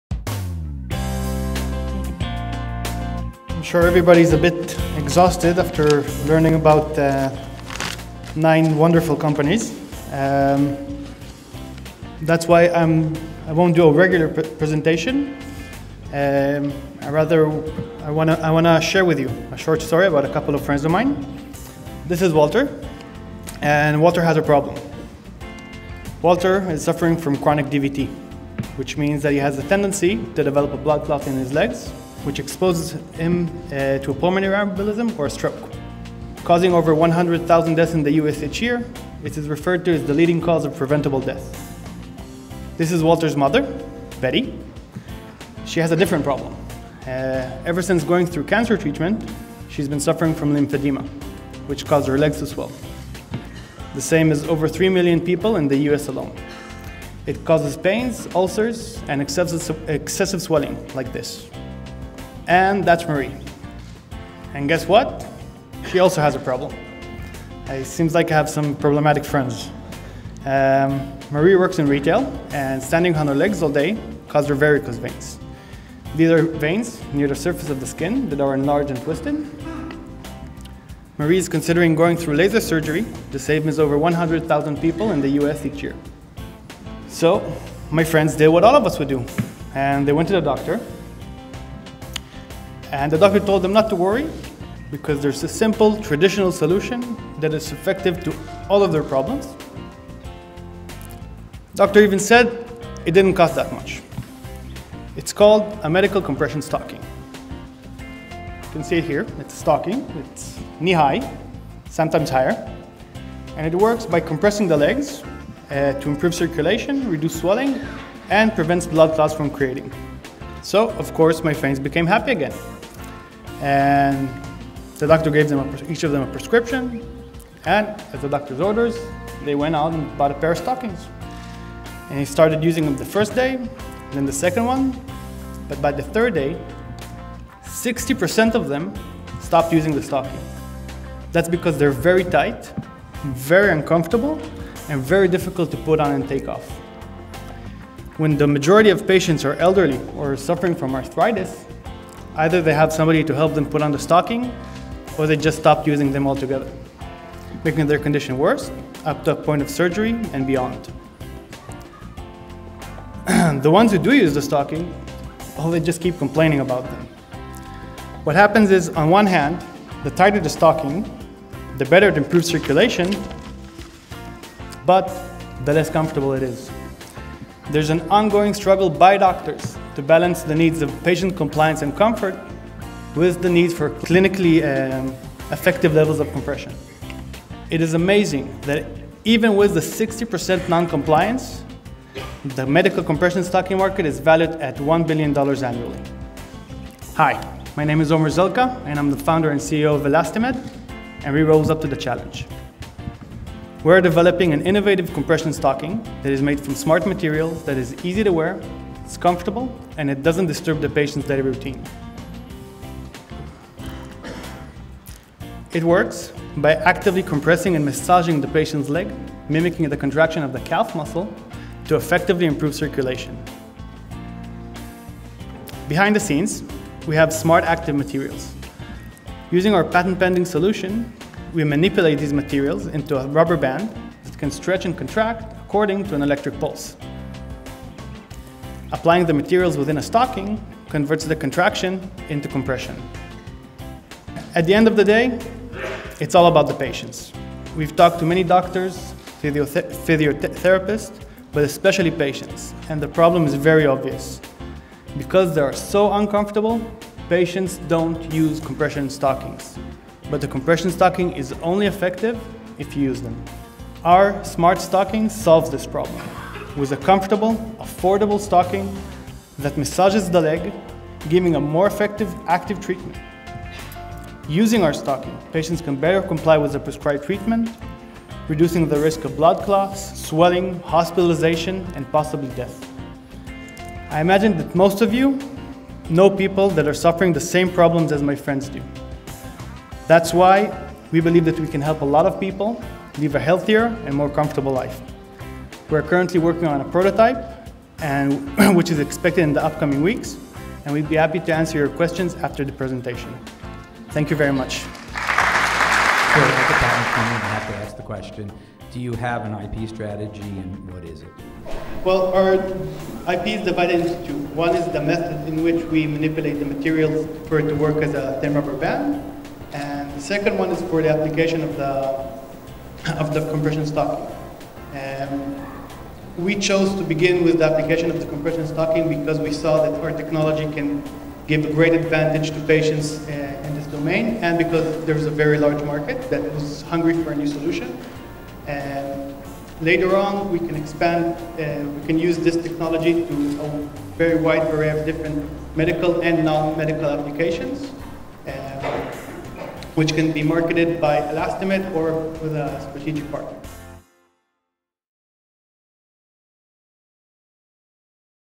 Easy-to-wear compression stocking from smart materials - ElastiMed
ElastiMed is developing a compression stocking made from smart materials that mimics rhythmic calf muscle contractions and stimulates blood flow.